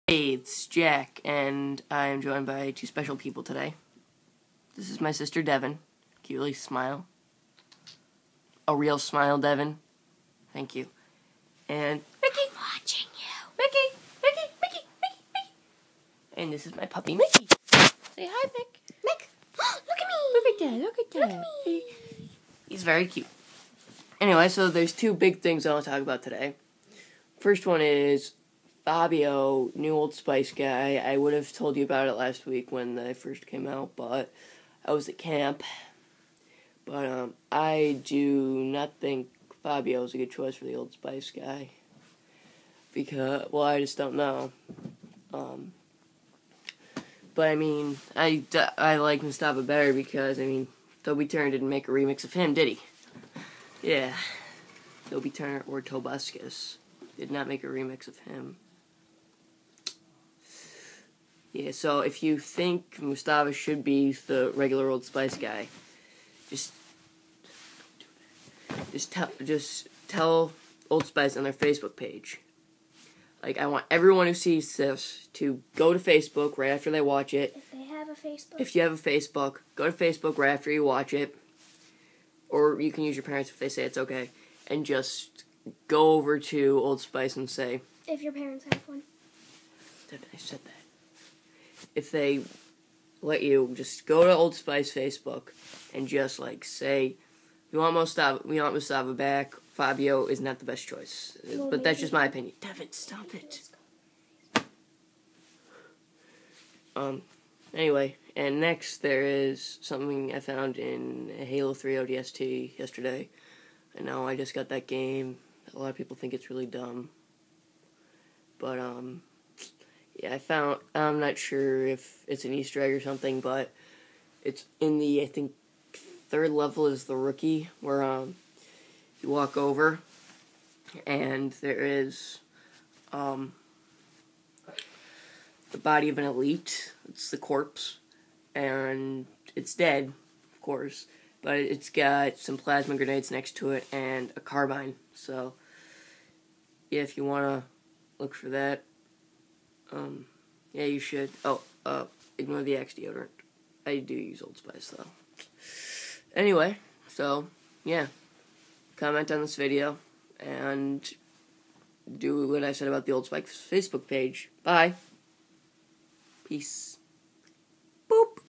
Roomvlog No. 5 Fabio vs Mustava for Old Spice guy and possi
Who do you think is a better old Spice guy Fabio or Mustava?  Tell it to me in the comments or Old Spice on Facebook